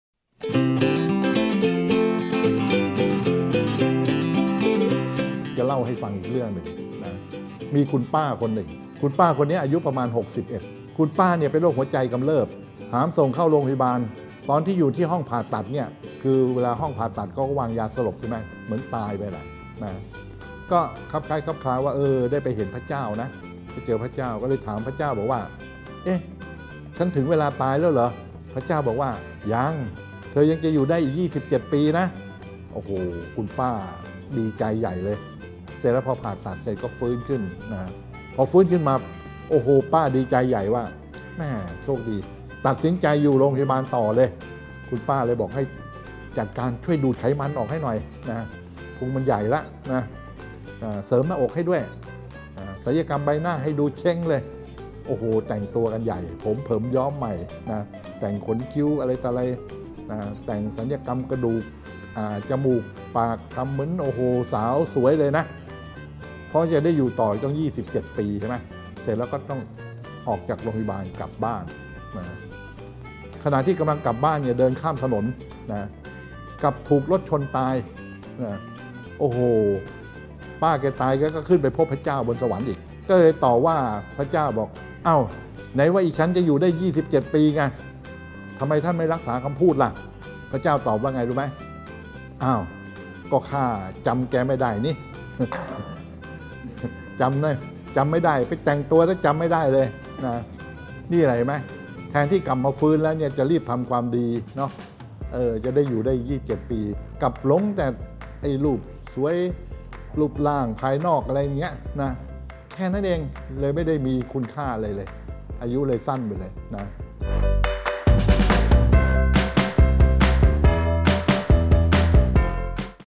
การ์ตูนง่ายๆ จดหมายจากพระเจ้า นพพล素晴らしい.(「アニメ」 anime)
การ์ตูน ประกอบธรรมะ เป็นคติสอนใจ.素晴らしい.(「アニメ」 anime)เป็นเรื่องของหญิงคนหนึ่งที่ประมาทในความตาย และความหลงกับการแต่งตัว จนถึงกับต้องตาย..